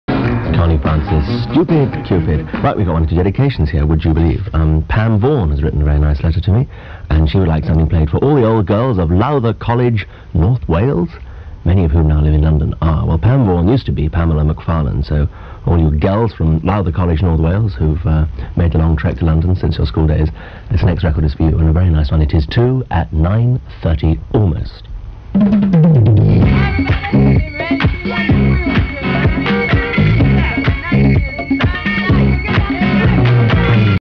London in 1978
This is a small part of an S8 film that Kurt Sellenthin (formerly Kristkowitz) made in 1978 when he visited London. The sound is from Capital Radio.